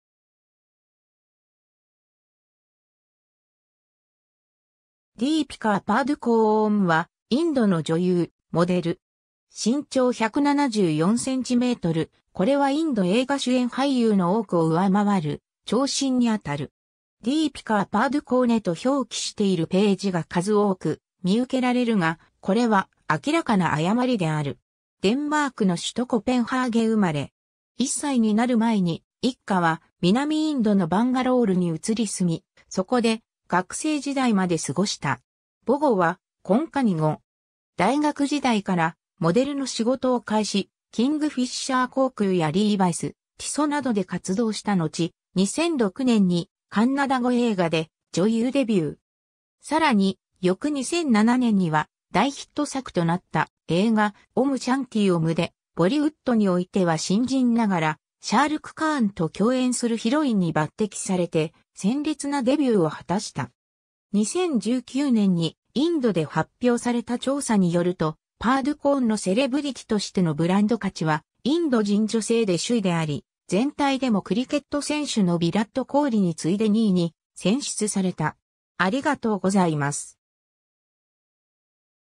ディーピカー・パードゥコーン
ジー・シネ・アワード受賞者
スター・スクリーン・アワード受賞者
ディーピカー・パードゥコーン（カンナダ語: ದೀಪಿಕಾ ಪಡುಕೋಣೆ、ヒンディー語: दीपिका पादुकोण、英語: Deepika Padukone、1986年1月5日 -）は、インドの女優・モデル。
身長174cm、これはインド映画主演俳優の多くを上回る長身にあたる。
ディーピカー・パードゥコーネと表記しているページが数多く見受けられるが、これは明らかな誤りである。
 デンマークの首都コペンハーゲン生まれ。
1歳になる前に一家は南インドのバンガロールに移り住み、そこで学生時代まで過ごした。
母語はコンカニ語。
 大学時代からモデルの仕事を開始、キングフィッシャー航空やリーバイス、ティソなどで活動したのち、2006年にカンナダ語映画で女優デビュー。
さらに翌2007年には大ヒット作となった映画『Om Shanti Om（恋する輪廻　オーム・シャンティ・オーム）』でボリウッド（北インド映画）においては新人ながら、シャー・ルク・カーンと共演するヒロインに抜擢されて鮮烈なデビューを果たした。
 2019年にインドで発表された調査によると、パードゥコーンのセレブリティとしてのブランド価値はインド人女性で首位であり、全体でもクリケット選手のヴィラット・コーリに次いで2位に選出された。